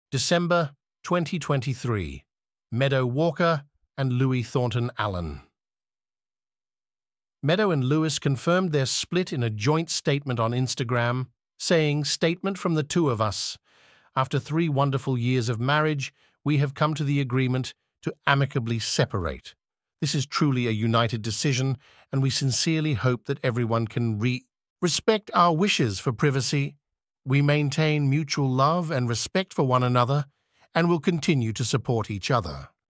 Meadow Walker and Louis Thornton-Allan separated
Meadow Walker and Louis Thornton-Allan have separated.